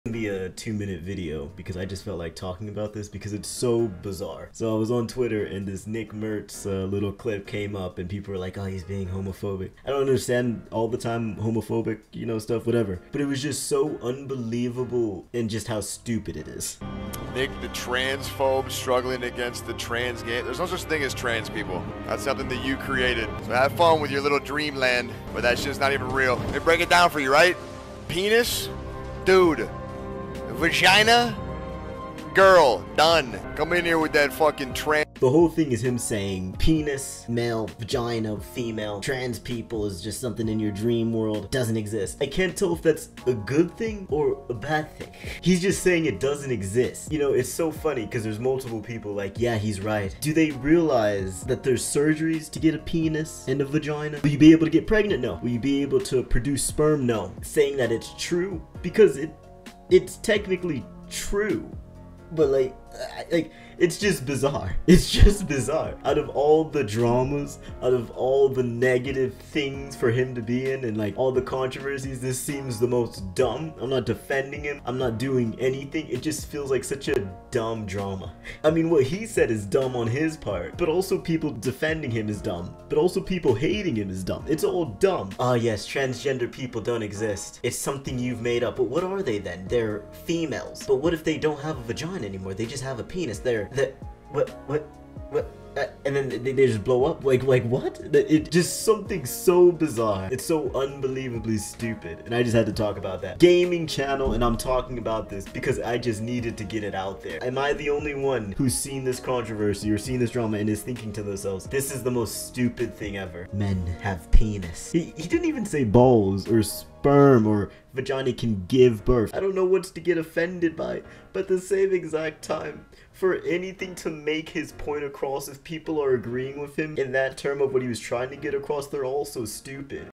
The Nick Mercs drama is bizarre
12 am and felt like making this real fast made me laugh at just the "drama". When people said it was something homophobic, I thought it was gonna be very messed up